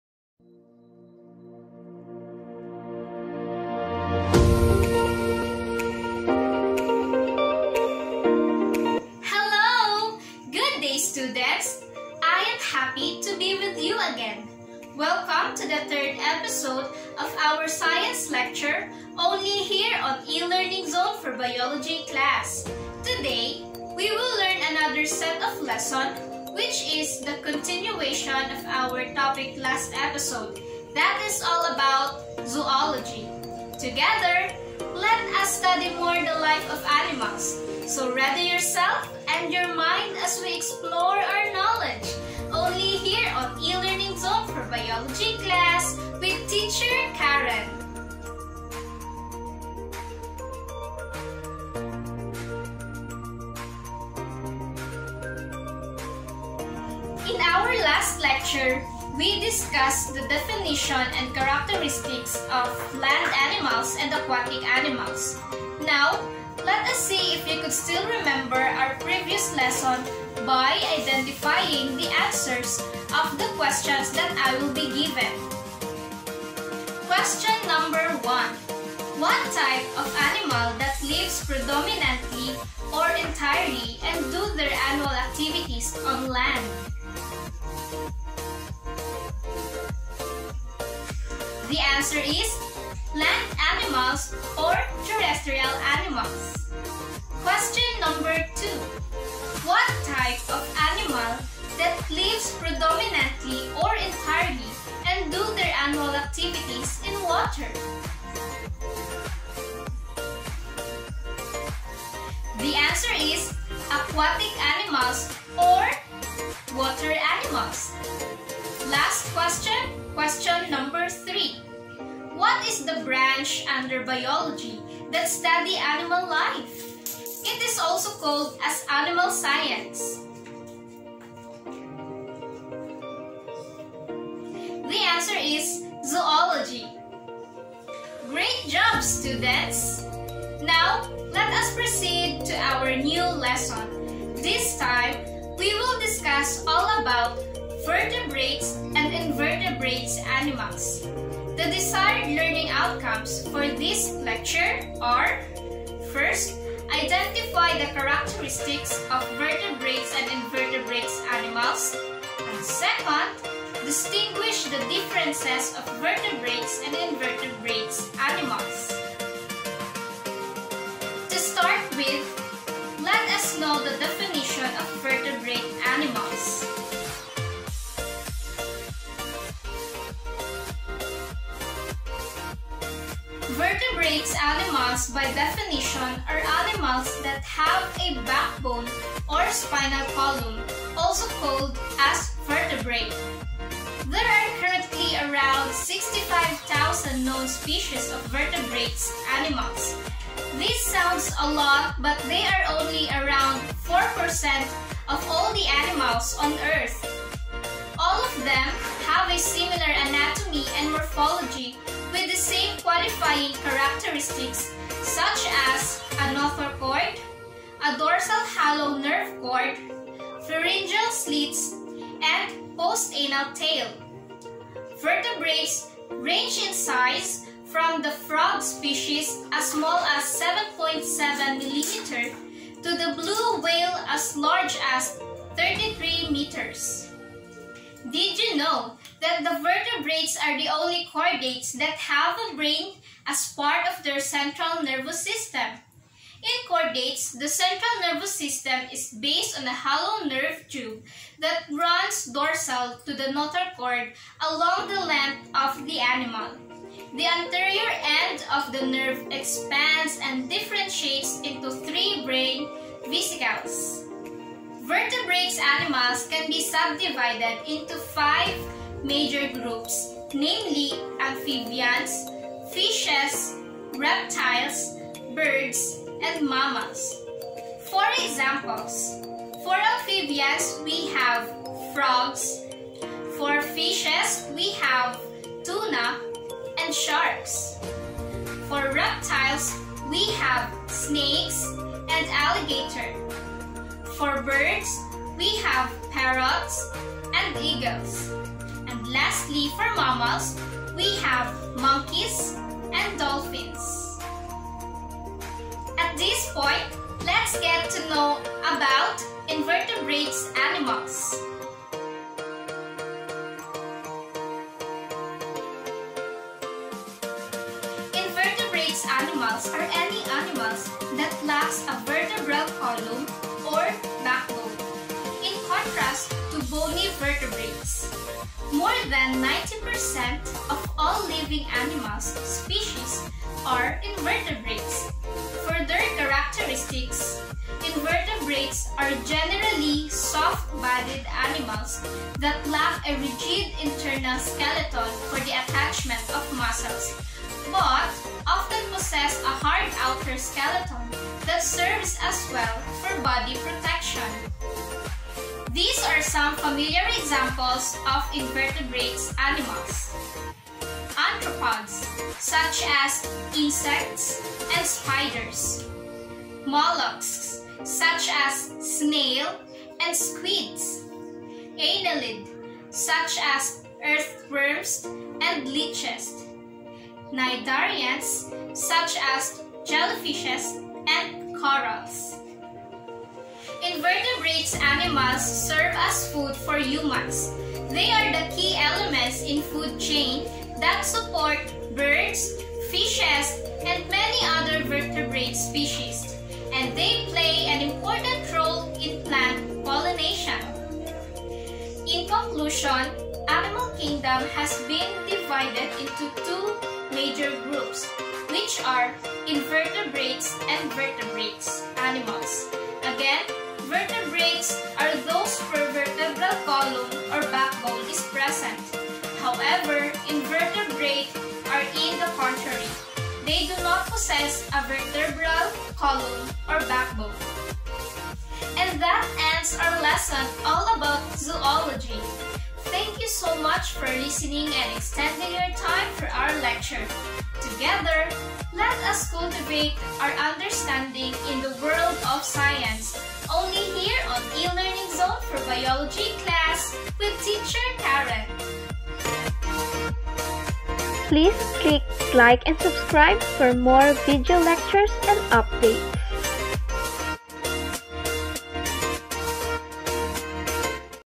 Vertebrates and Invertebrates Animals
Animal kingdom ahs been divided into two major groups: invertebrates and vertebrates. Vertebrates are those where vertebral column or
backbone is present, while invertebrates are in the contrary, do not possess a vertebral column or backbone.

This video lecture will give you additional information all about these two major groups of animals. Together, let us learn science only here on E-learning Zone for biology class with teacher Caren!